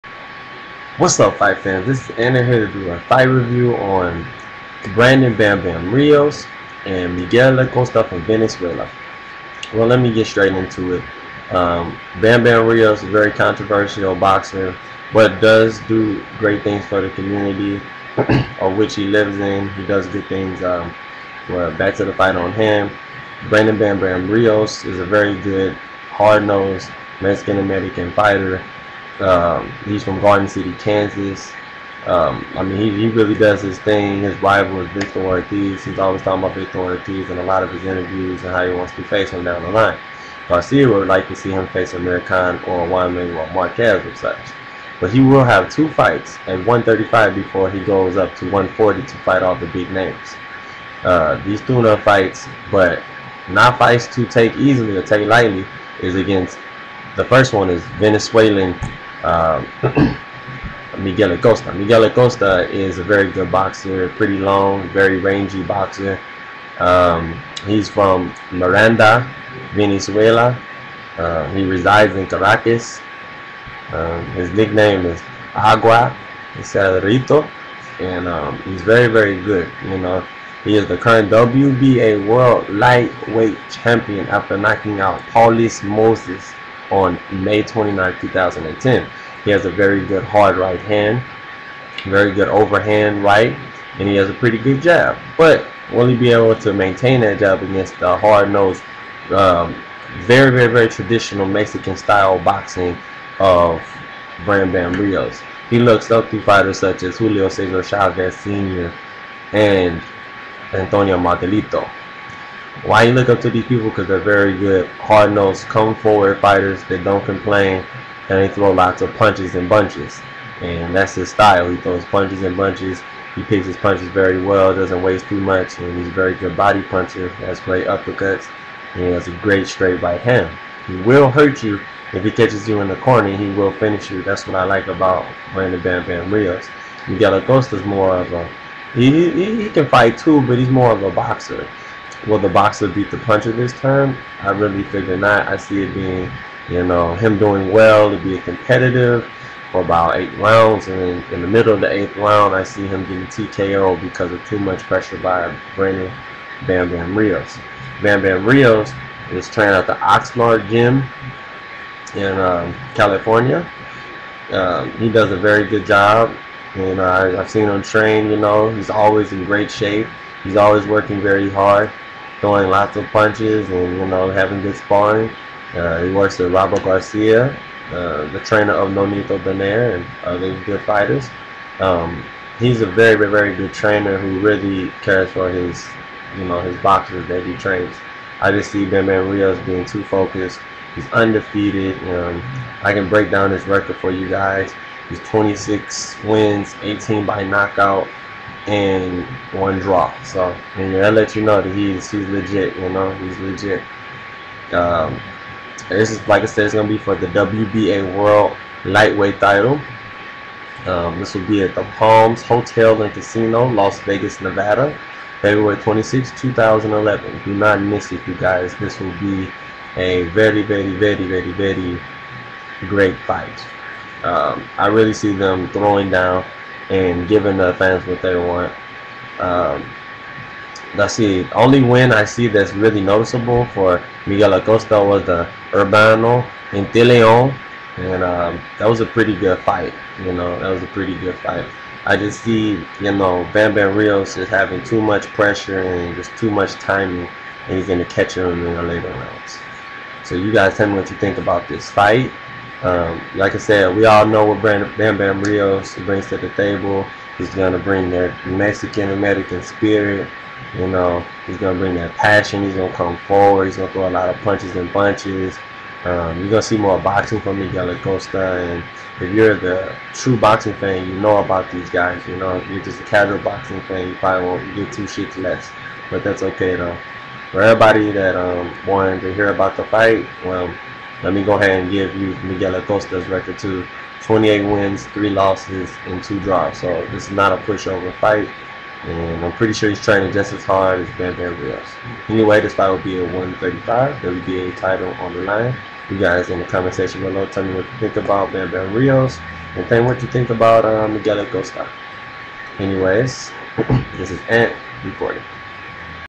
BRANDON "BAM BAM" RIOS VS MIGUEL ACOSTA ANALYSIS
8TH ROUND KO TOO MUCH PRESSURE FROM BAM BAM RIOS